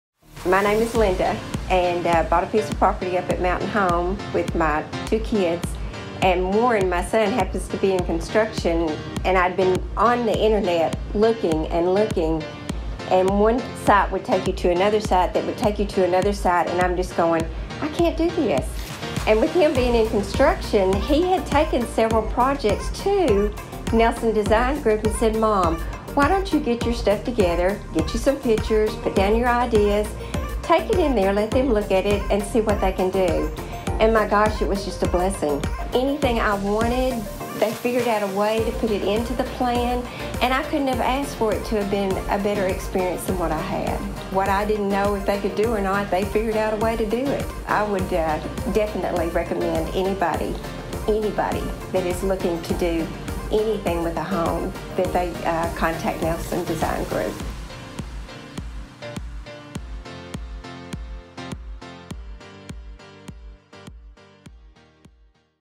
Linda's Story
"I couldn't have asked for a better experience than what I had" - Linda

We had the incredible opportunity helping Mrs. Linda design her dream home. She didn't know where to start, that is until she came to Nelson Design Group. We helped her from step 1 (finding the perfect Home Plan) to step DONE.

It's more than just a home, it's a lifestyle!